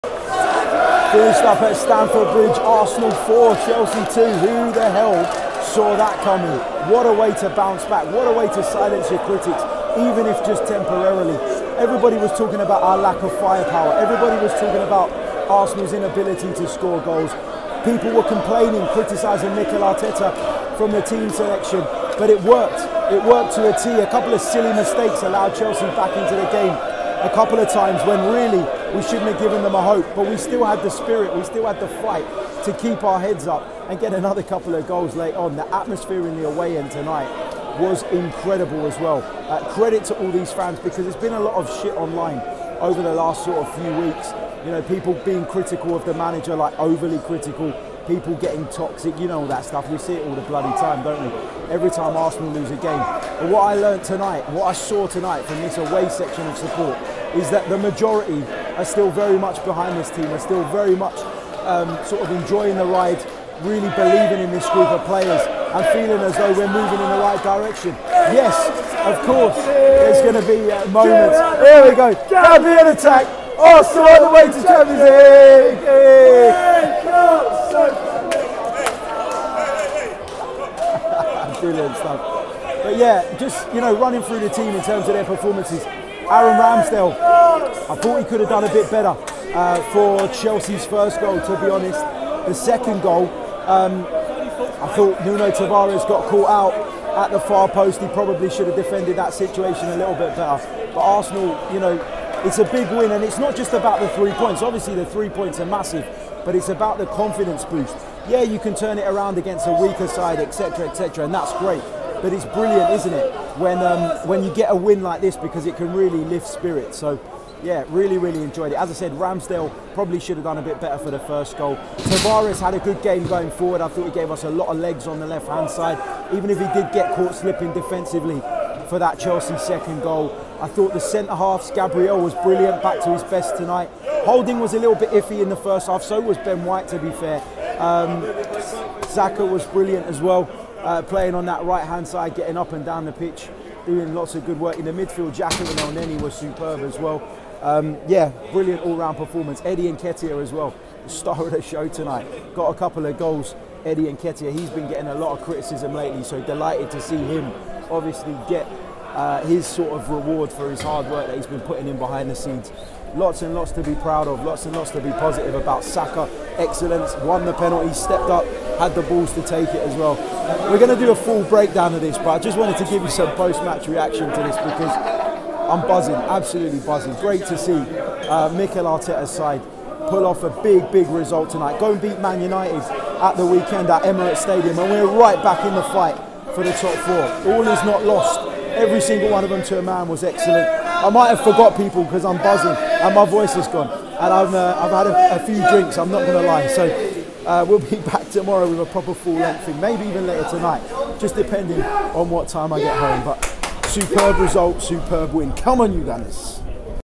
Chelsea 2-4 Arsenal | Reaction
Harry Symeou reacts to Chelsea 2-4 Arsenal in the Premier League